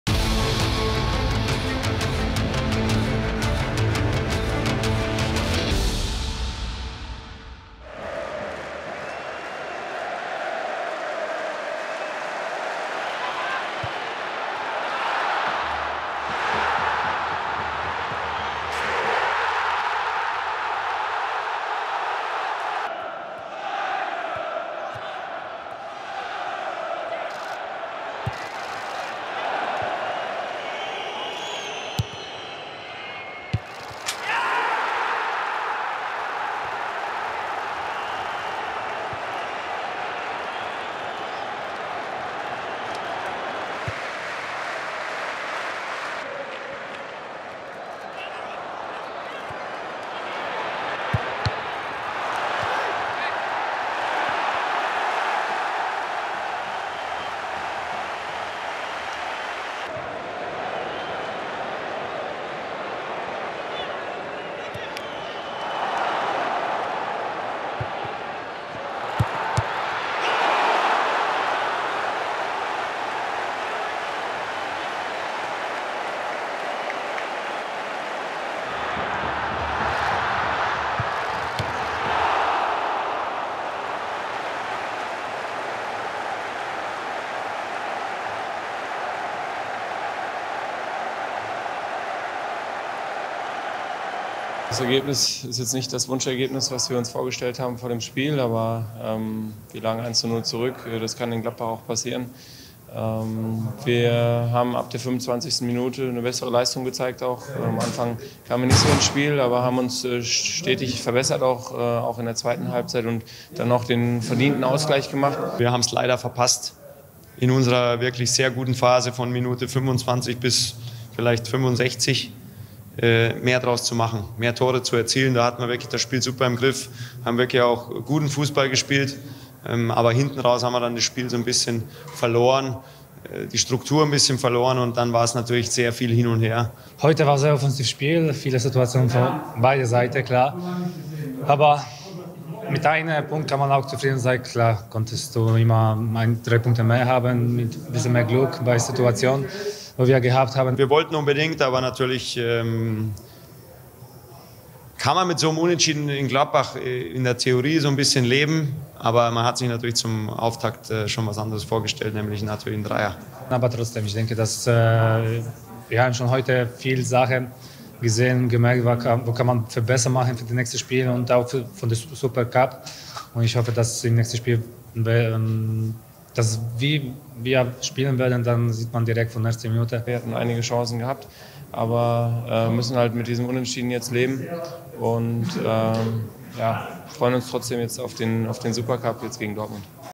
Lewandowski saves the draw | Highlights Borussia Mönchengladbach vs. FC Bayern
FC Bayern Munich gets a 1-1 draw against Borussia Mönchengladbach in the Bundesliga opening match. Check out the highlights of first match of the season!